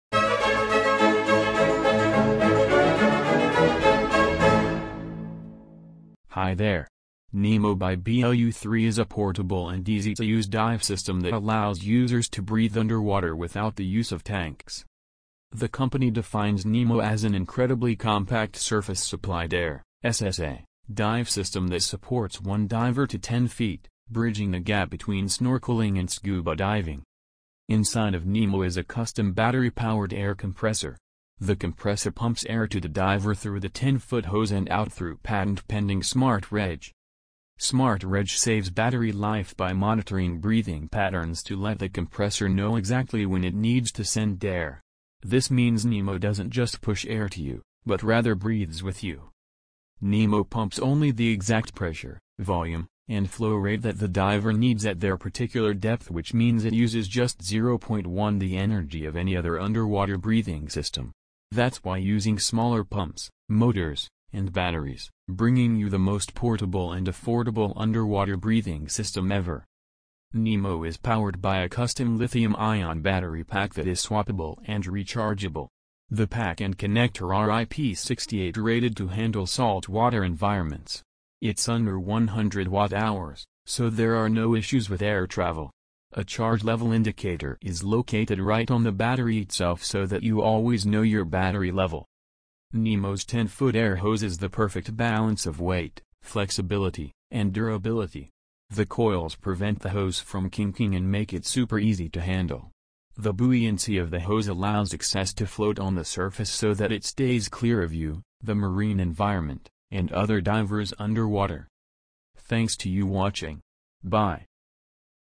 Blu3 claims world's smallest, lightest Surface-Supplied Air dive system
NEMO is the perfect way for snorkelers to take their adventures beyond the surface. It's an incredibly compact Surface-Supplied Air dive system that supports one diver to ten feet.